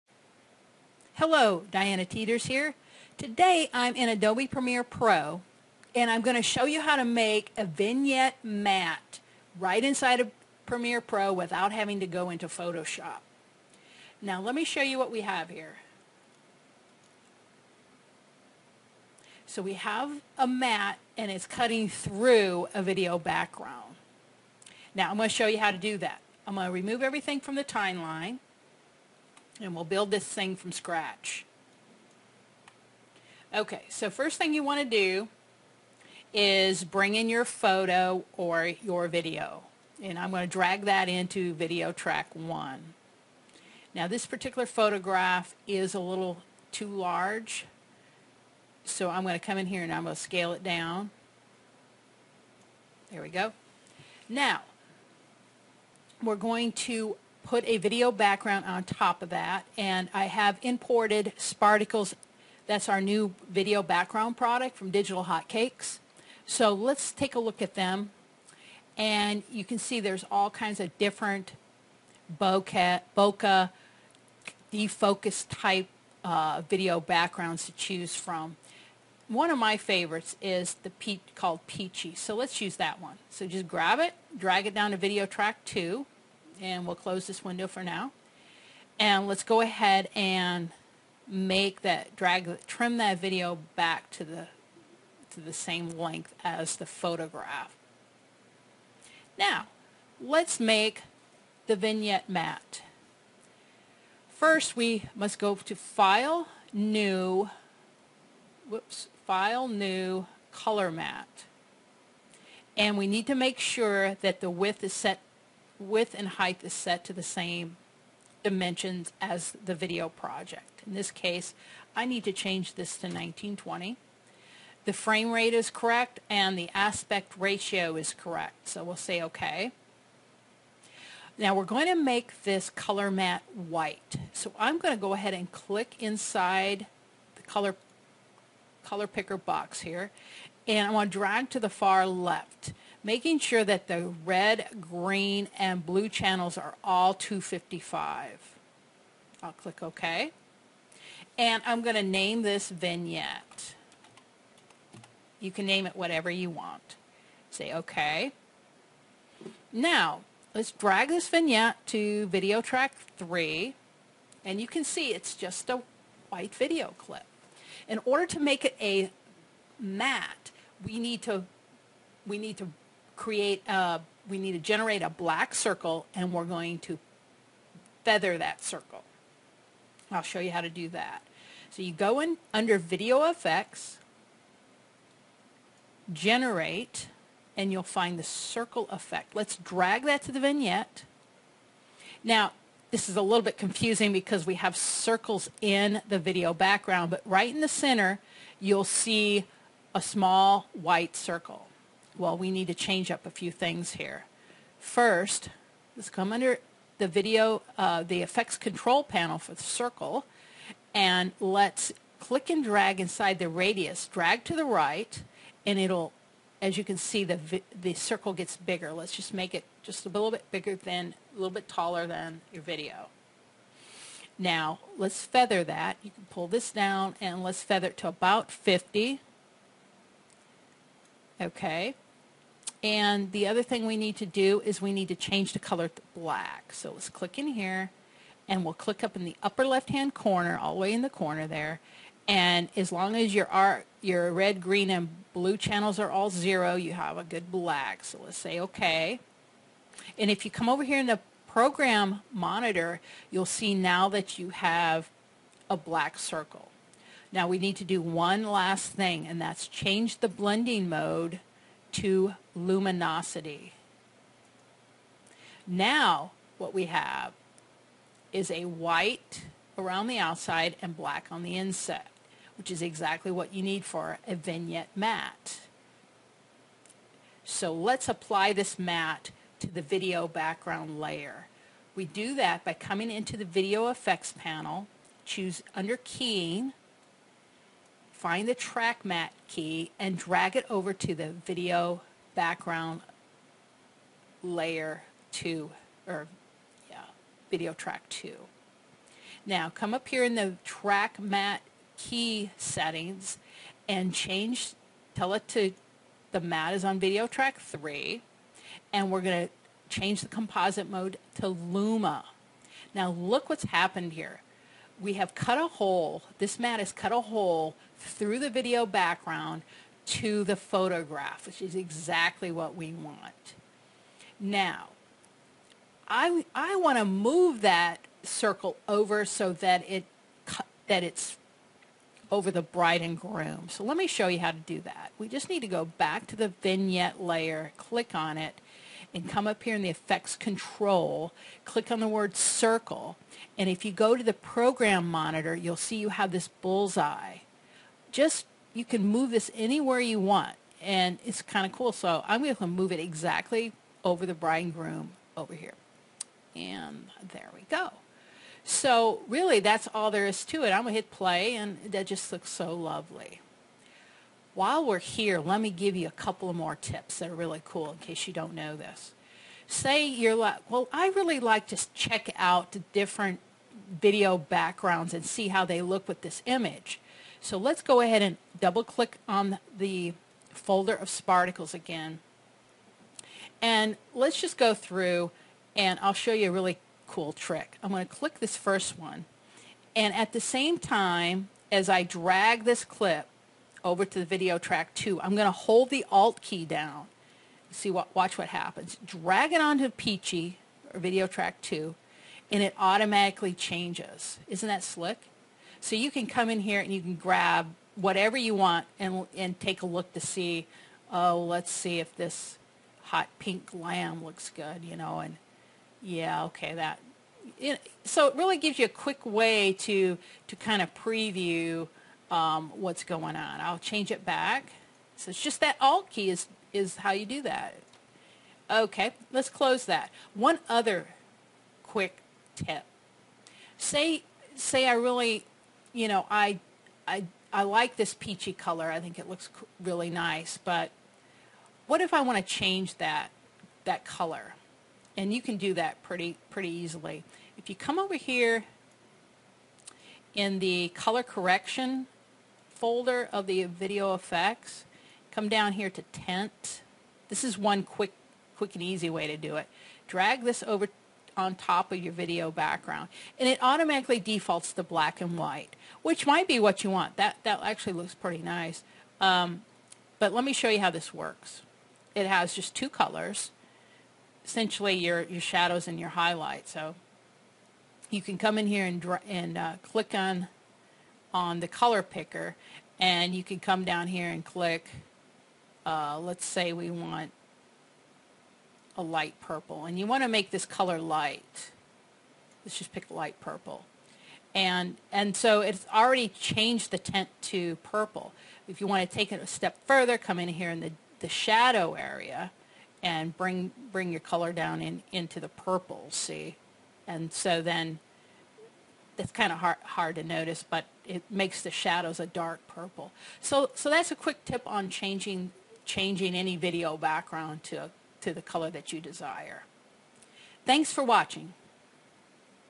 How to Make a Vignette Track Matte in Premiere Pro and Apply to a Video Background
See a short tutorial video on creating a vignette matte inside Adobe Premiere Pro. No Photoshop required. Then see how to use the vignette as a track matte with Digital Hotcakes, Sparticles video backgrounds. Bonus tip you won't want to miss at the end!